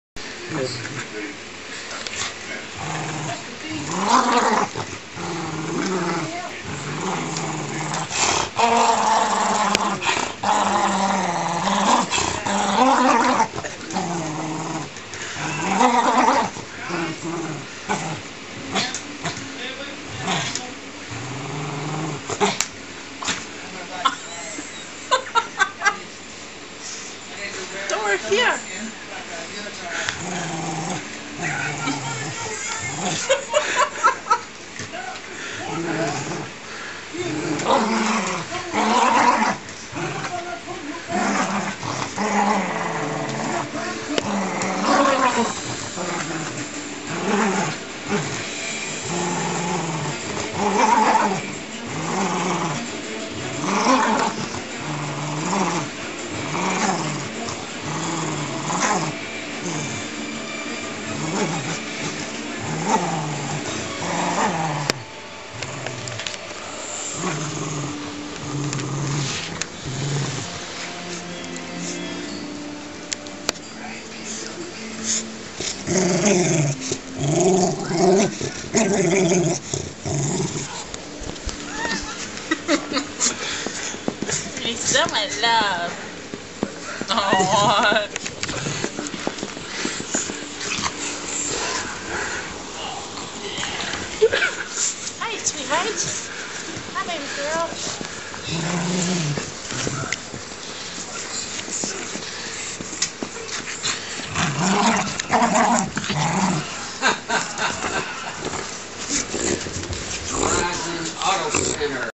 Thor playing tug-o-war!
Thor the dachshund playing tug-o-war with one of his gutted toys (he rips the stuffing out of everything lol), also featuring Earnie the weenie! she had to get in on the action.